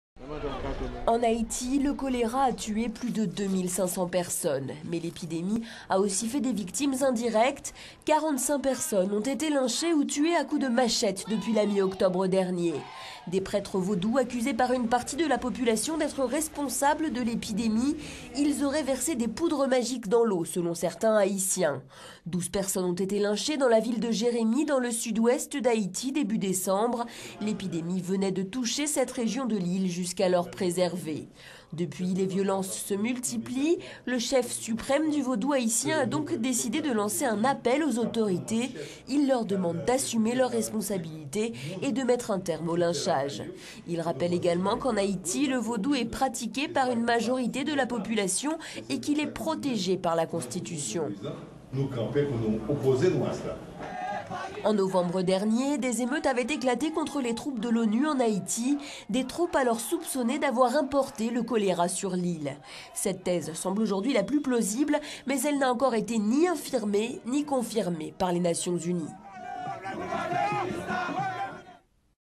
Haïti: des prêtres vaudous lynchés, accusés d'avoir...
En Haïti, le cholera a tué plus de 2500 personnes.

Mais l'épidémie a aussi fait des victimes indirectes: 45 personnes ont été lynchées, ou tuées à coups de machettes depuis la mi-octobre dernier.

Il s'agît de prêtres vaudous accusés par une partie de la population d'être responsables de l'épidémie.

Ils auraient versé des poudres magiques dans l'eau et jeté des mauvais sorts, selon certains Haïtiens.